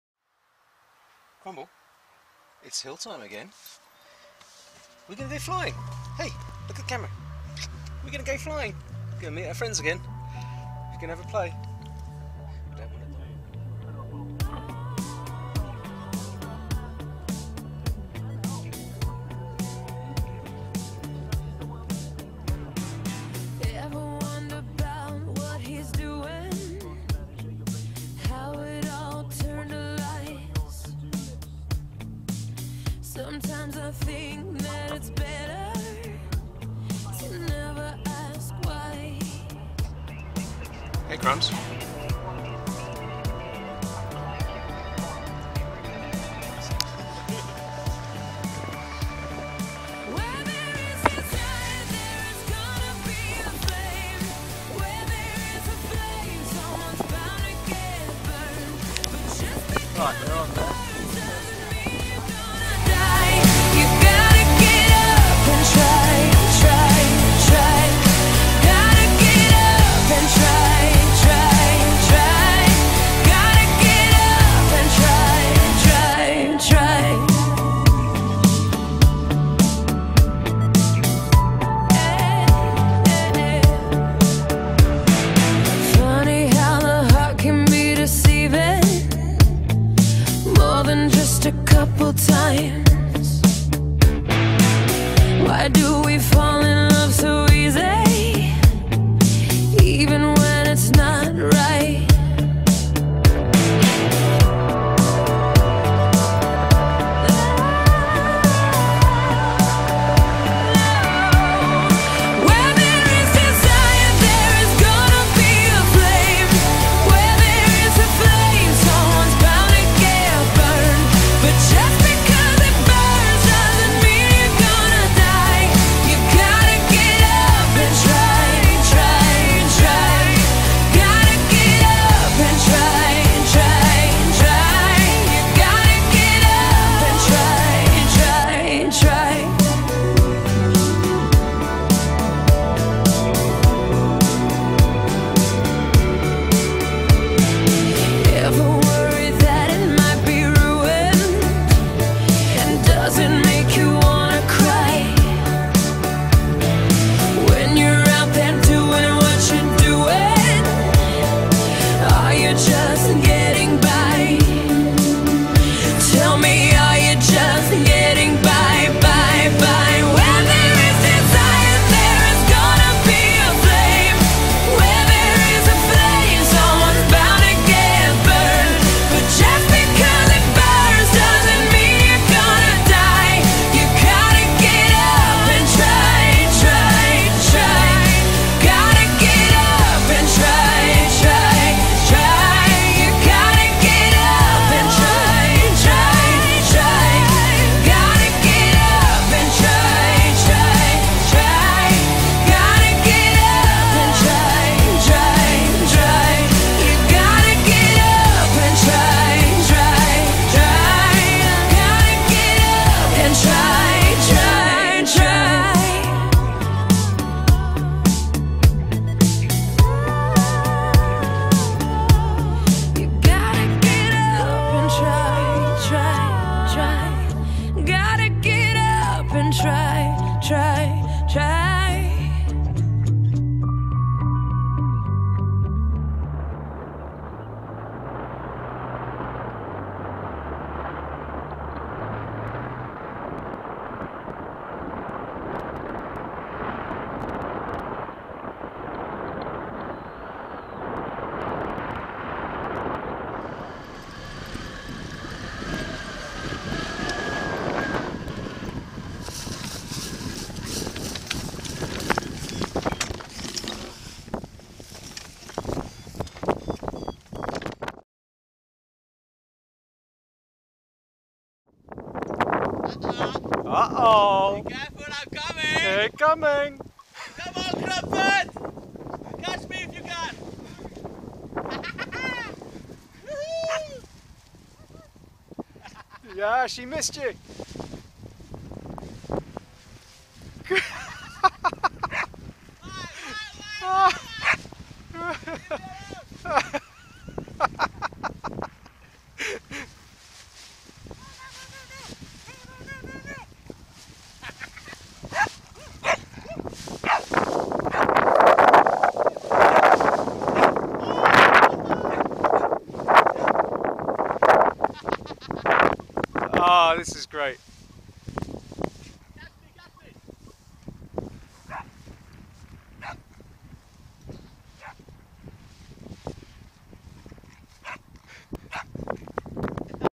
Tricky Truleigh
12th November 2013. After a morning of drizzle the afternoon promised some good flying at Devils Dyke. It never quite lived up to that with no one getting